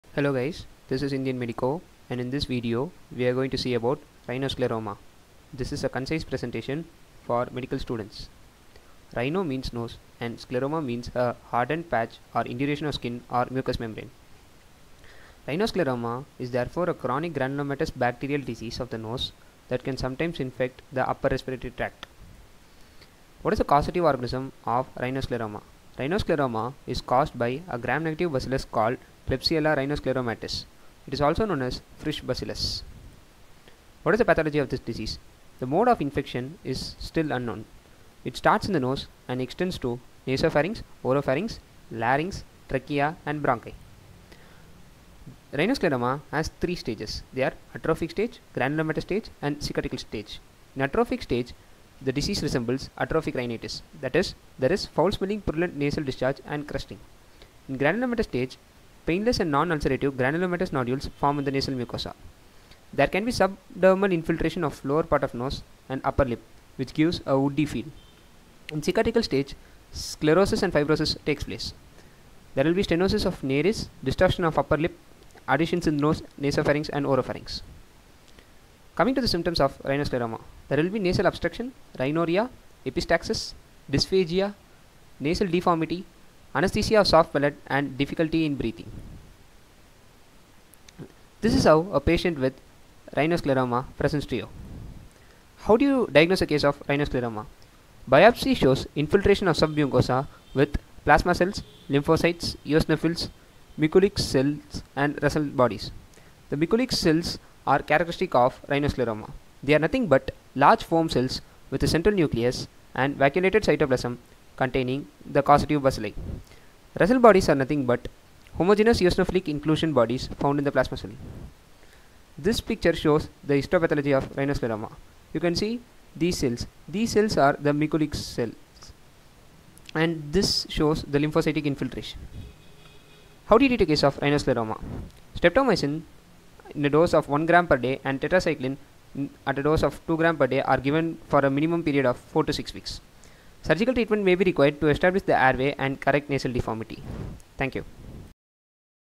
Rhinoscleroma - ENT
Hey guys, this is Indian Medico. In this video, we are going to see about Rhinoscleroma. This is a concise presentation for medical students (especially from India). I hope that this presentation helps you to understand the topic as well as score well in your exams. I welcome your feedback. If you don't like this video, hit the dislike button and comment your suggestions please.